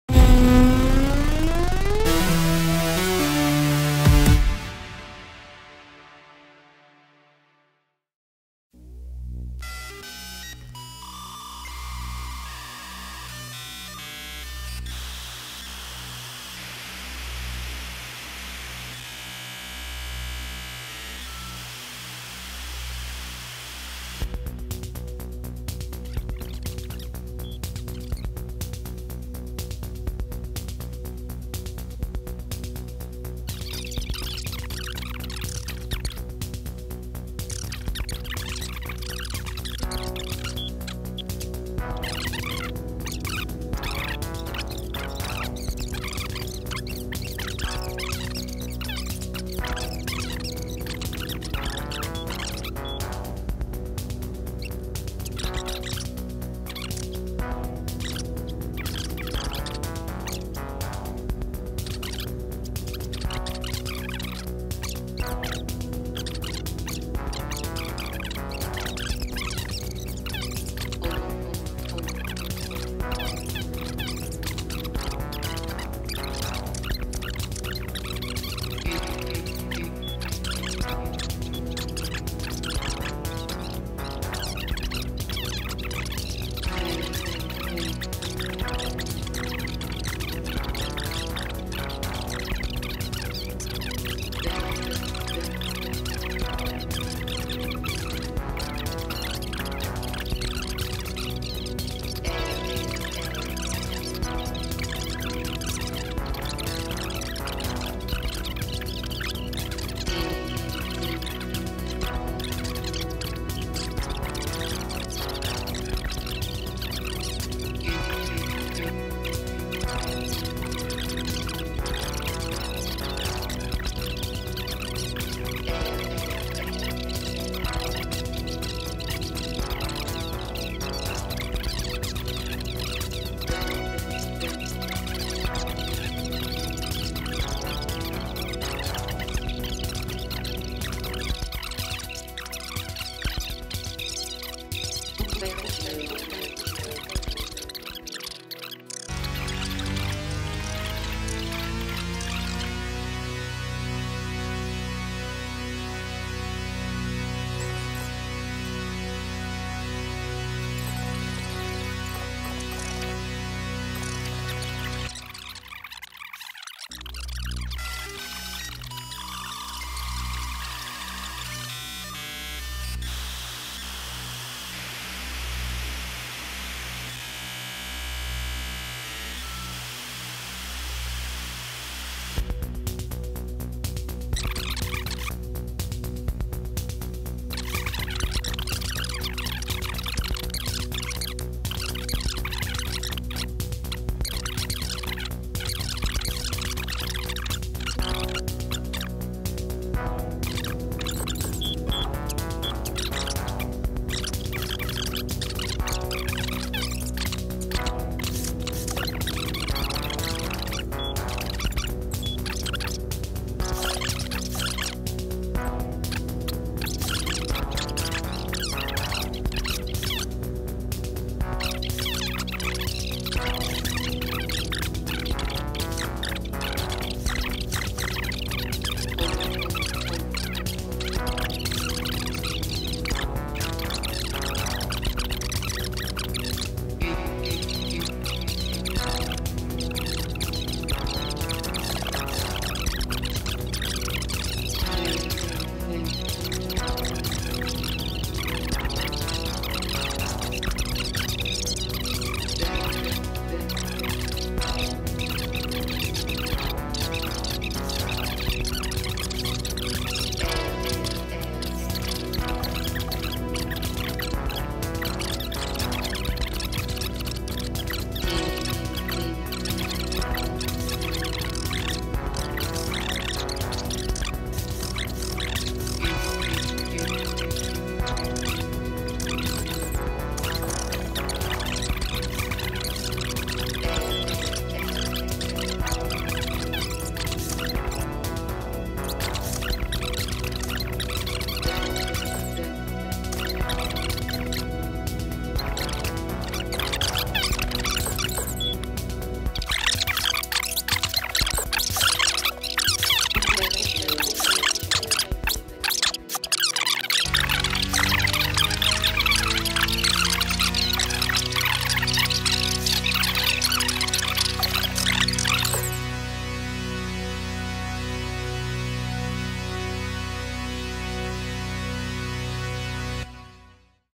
Come si monta Bruno Power (tutorial) - ASGANAWAY
Wender spiega come si realizza la confezione di una puntata di Bruno Power!